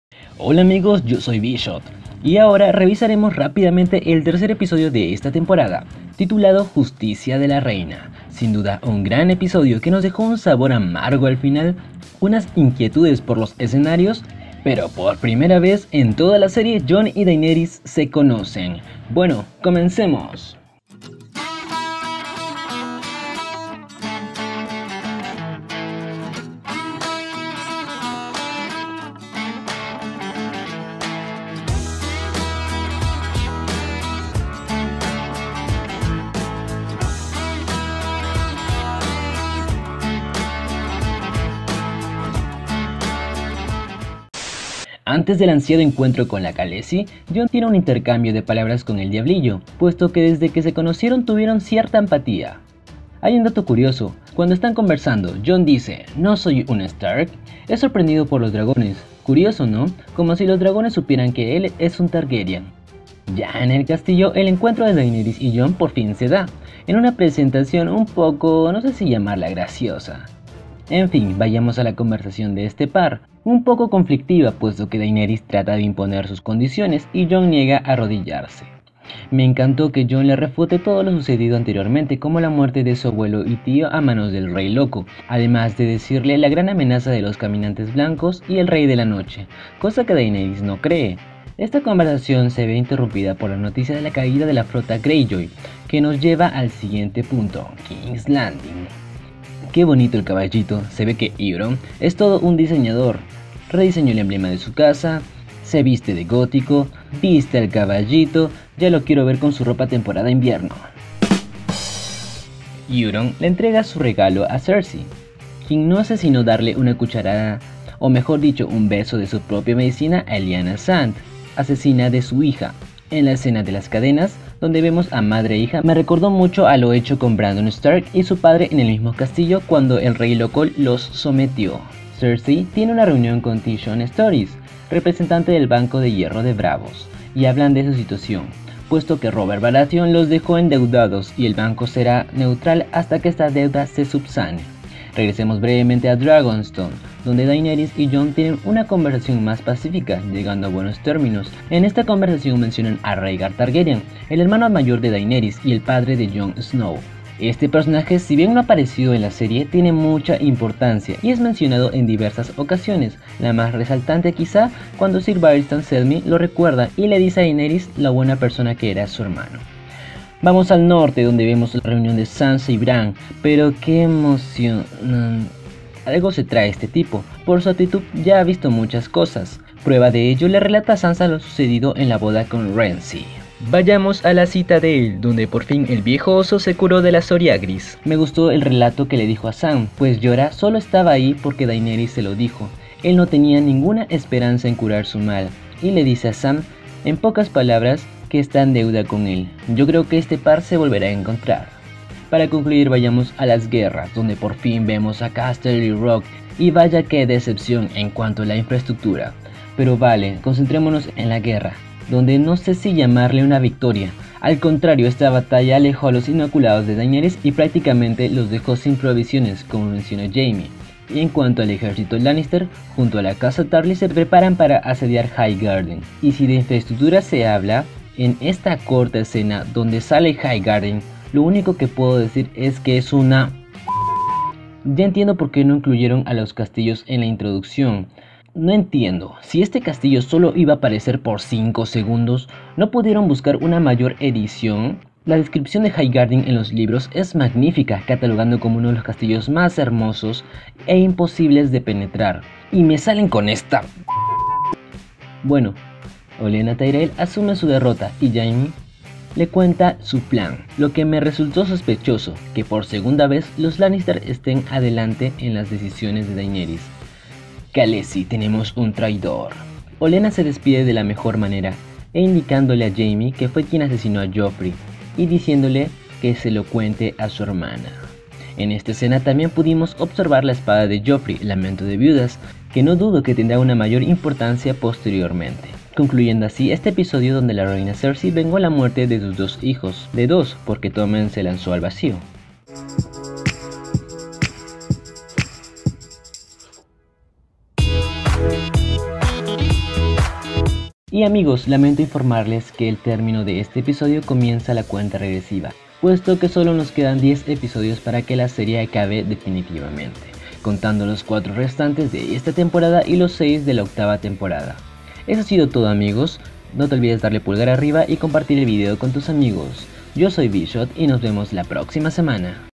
REVIEW Juego de Tronos 7x03 - La justicia de la reina
hola amigos les traigo un nuevo video, mi análisis del 7x03 de Juego de Tronos.